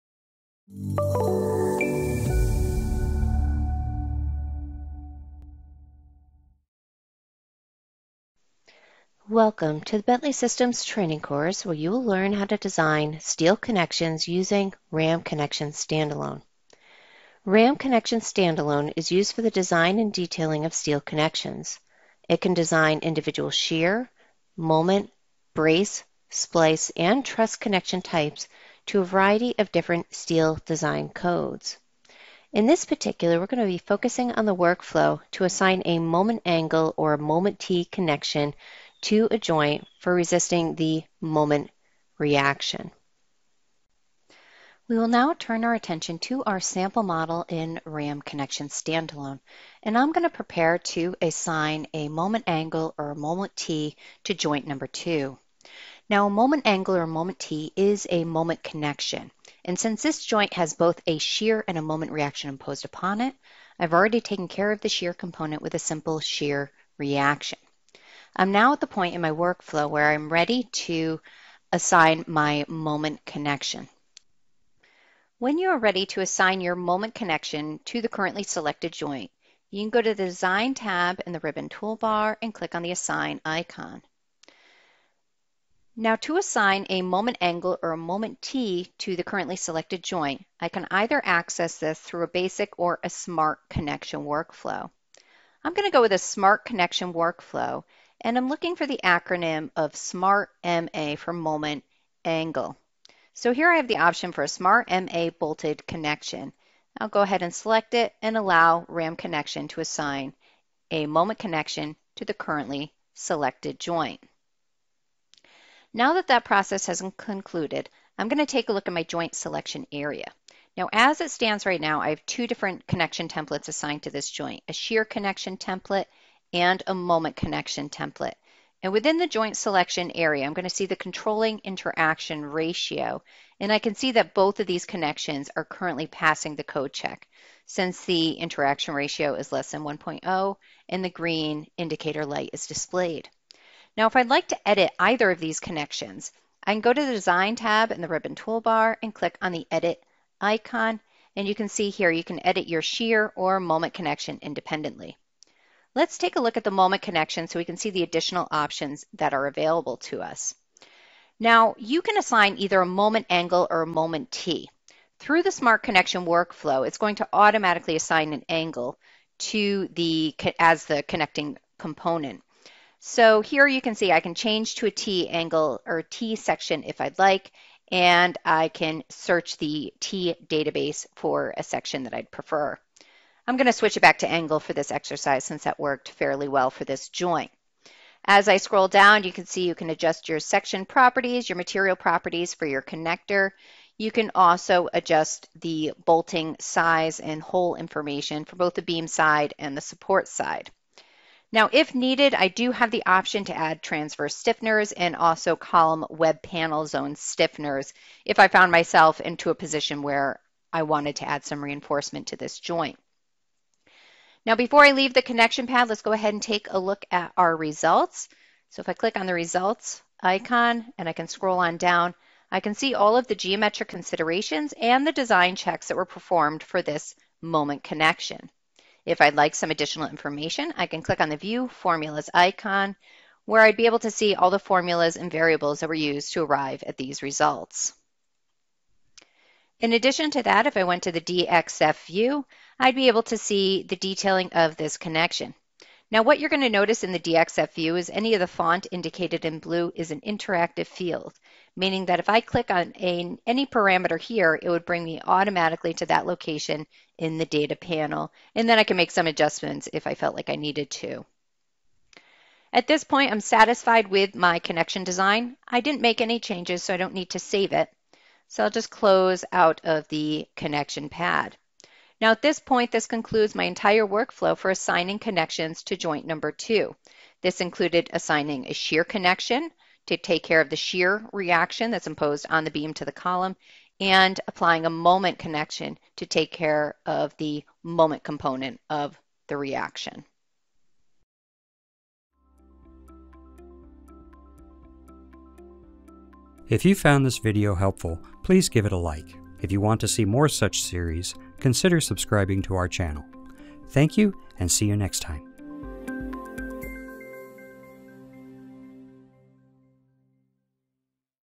Designing Moment Angle/Tee (MA or MT) Connections in RAM Connection
In this video, you will learn how to design moment angle/moment tee moment connections for beam to column joints.